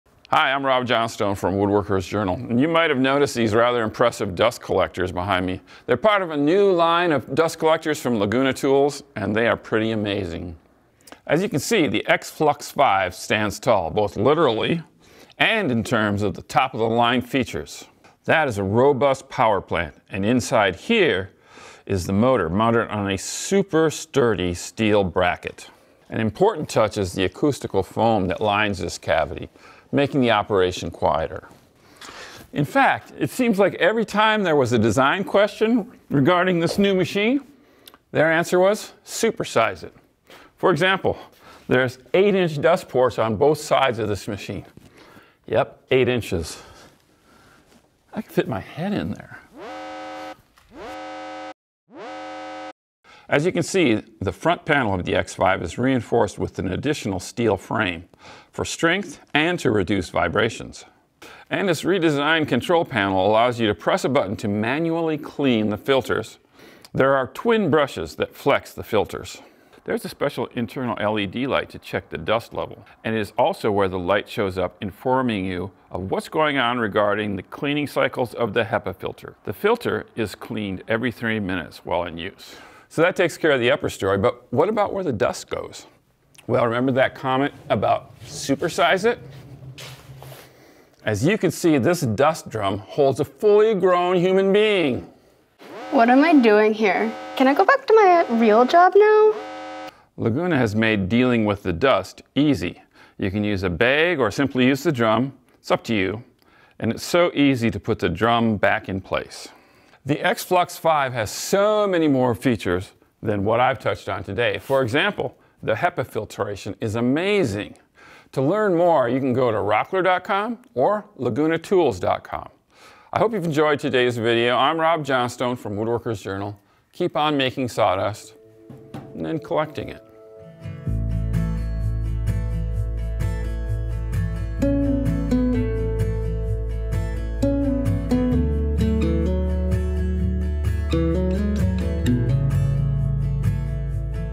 Laguna X | Flux : 5 5HP 220V HEPA Cyclone Dust Collector Overview
The all-new X|Flux 5 boasts a powerful 5HP motor and moves an impressive 1823 CFM of air. Auto-cleaning of the HEPA filter is accomplished with motorized dual paddles that sweep the filter every 3 minutes in a clockwise and counterclockwise direction. A handheld remote and LED drum light come standard. Perfect for the demanding hobbyist or small pro shop. The X|Flux 5 lets you run a large-diameter pipe through your shop with drops to individual machining stations. It features full HEPA filtration for the most pristine shop air, and a convenient 39 gallon drum that can be lowered onto locking casters for emptying.